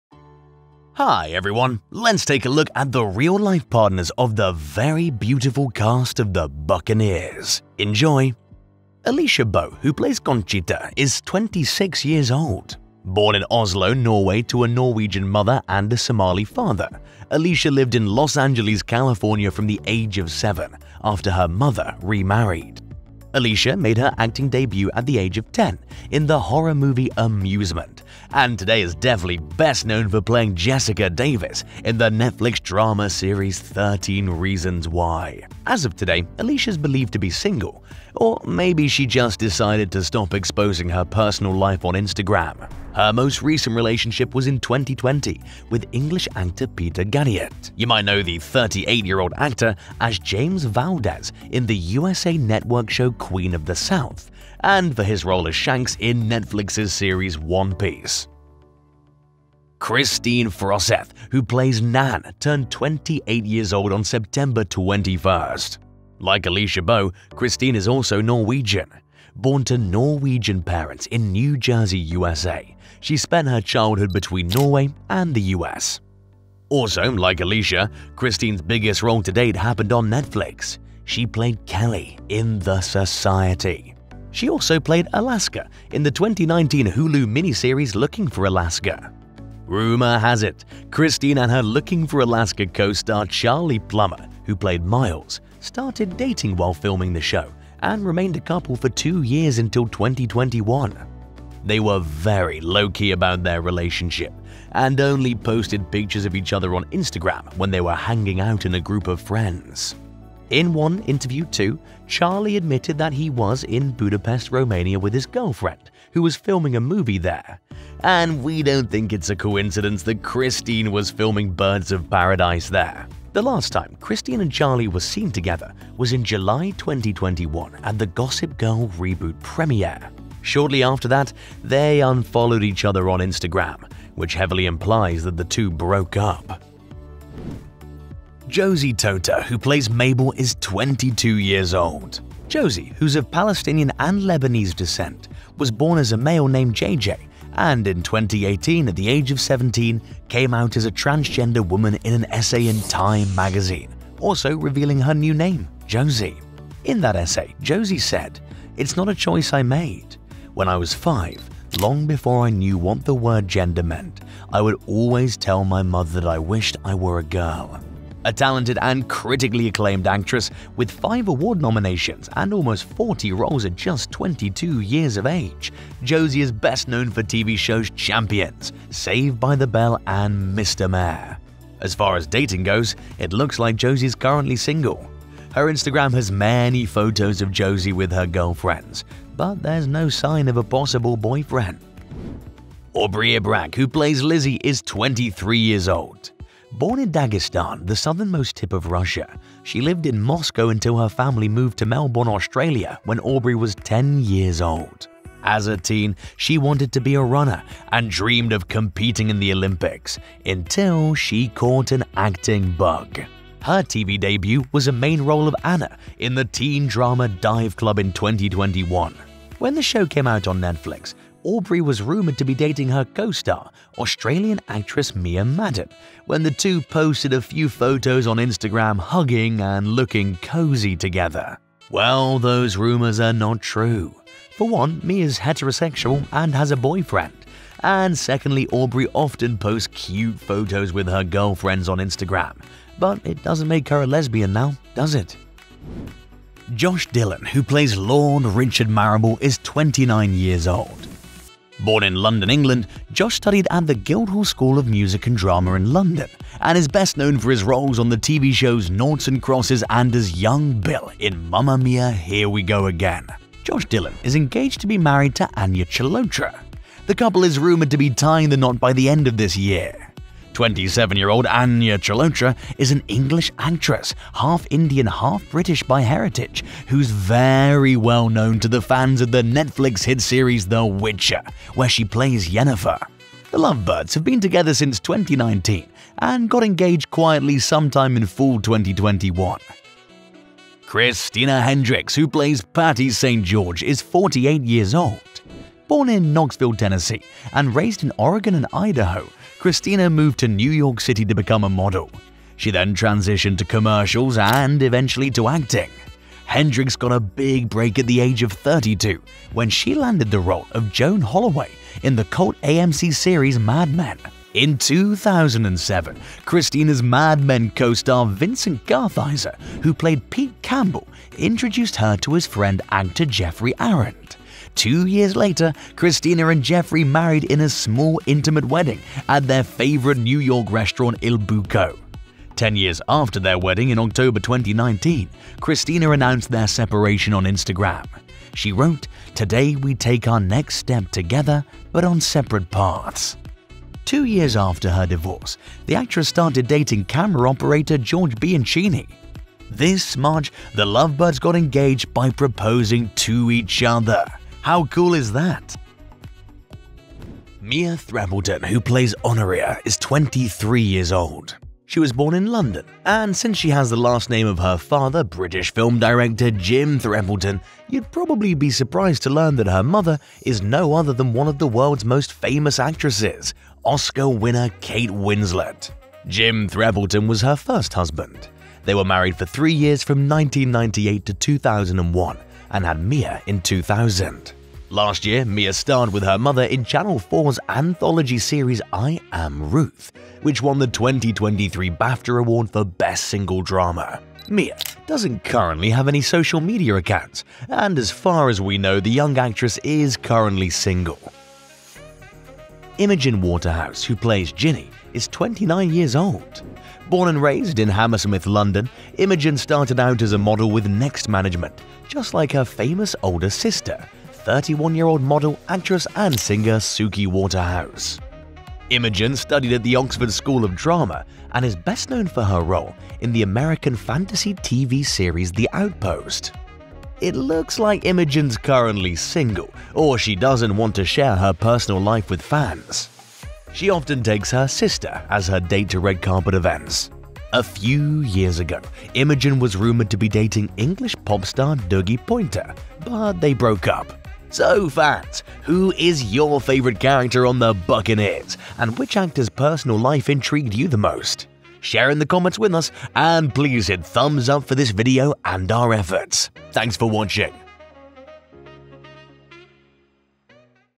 THE BUCCANEERS Real-Life Couples ❤️ Alisha Boe, Kristine Froseth, Josie Totah Aubri Ibrag Josh Dylan
THE BUCCANEERS actors real-life partners 2023:
0:00 Alisha Boe, who plays Conchitta
1:00 Kristine Froseth, who plays Nan
2:17 Josie Totah, who plays Mabel
3:14 Aubri Ibrag, who plays Lizzy
4:07 Josh Dylan, who plays Lord Richard Marable
4:51 Christina Hendricks, who plays Patti St. George
5:59 Mia Threapleton, who plays Honoria
6:42 Imogen Waterhouse, who plays Jinny